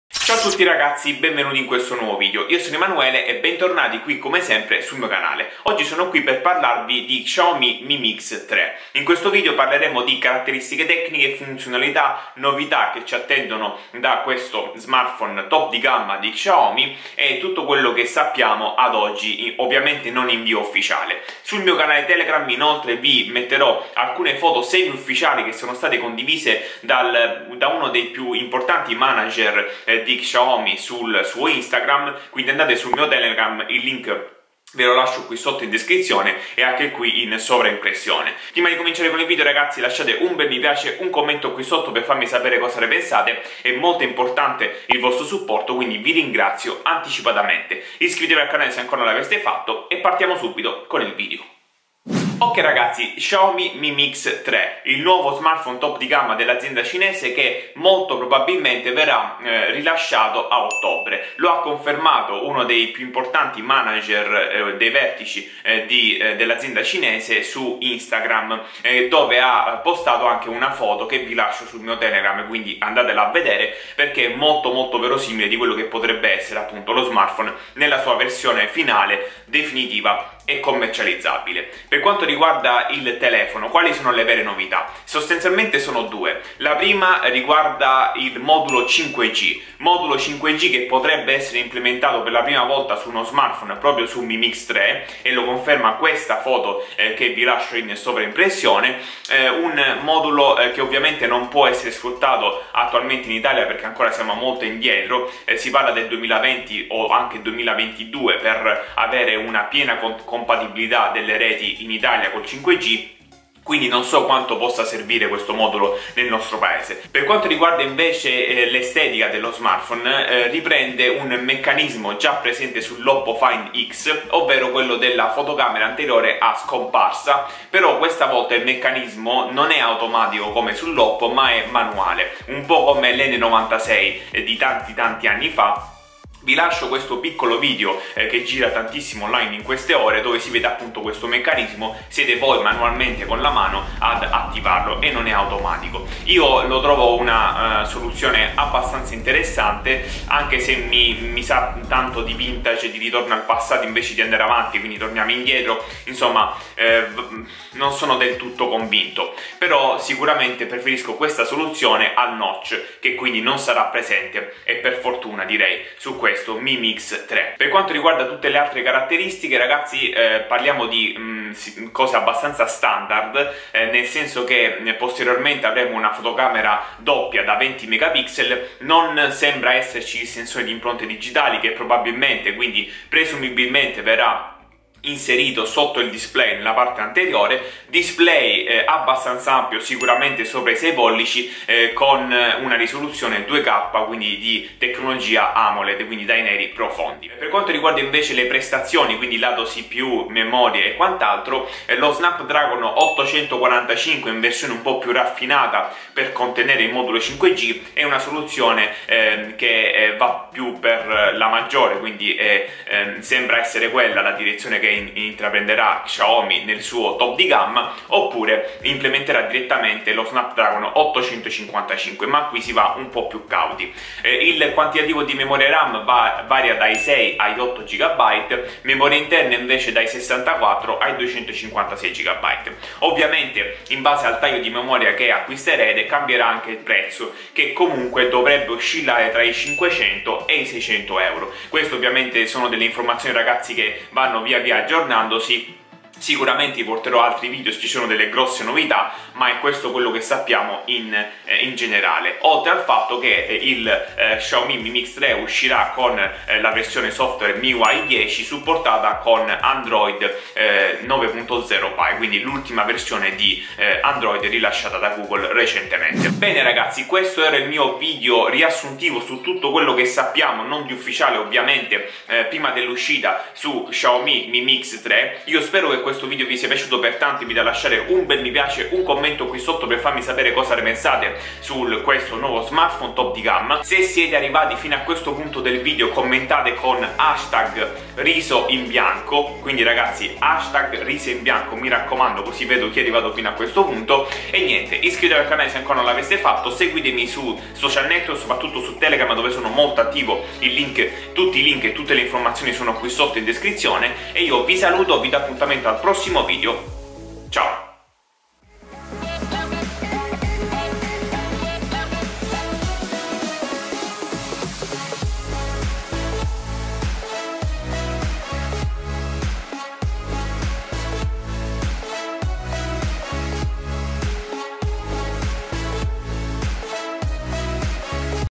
XIAOMI MI MIX 3: novità, caratteristiche, prezzi, uscita | ITA
Salve gente, oggi vi parlo del nuovo Xiaomi Mi Mix 3. Vediamo insieme quelle che sono tutte le novità e i rumors che conosciamo riguardo questo nuovo smartphone molto atteso. In questi giorni si susseguono rumors riguardo le caratteristiche, il design, la scheda tecnica, i prezzi e la data di uscita: ecco tutto quello che dovete sapere!

Cosa ne pensate di Xiaomi Mi Mix 3? Fatemelo sapere con un bel commento qui sotto :)

🎥 LA MIA ATTREZZATURA ➜

🎵 MUSICA ➜

✔️ HAI VISTO ➜XIAOMI MI MIX 3: novità, caratteristiche, prezzi, uscita | ITA